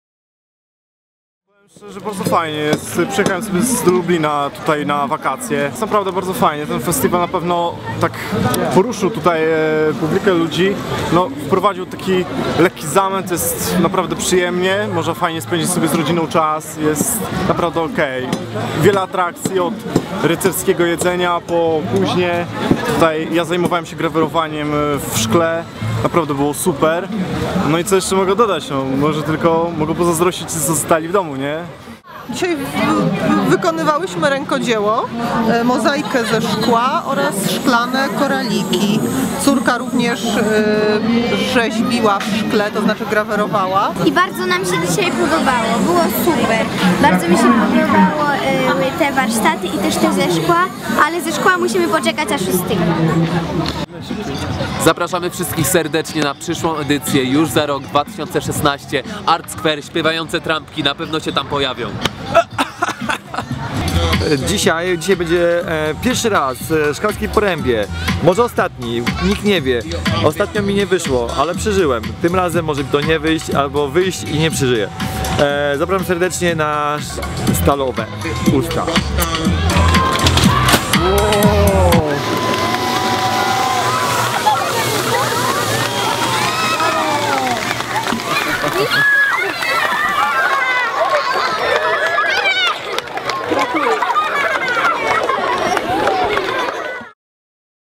Zakończenie Letniego Festiwalu Sztuki Art Skwer Szklarska Poręba
Letni Festiwal Sztuki to wakacyjny cykl warsztatów, pokazów i występów. To już szósty rok przygotowujemy ofertę kulturalną dla mieszkańców i turystów we wszystkich grupach wiekowych. Zapraszamy w 2016!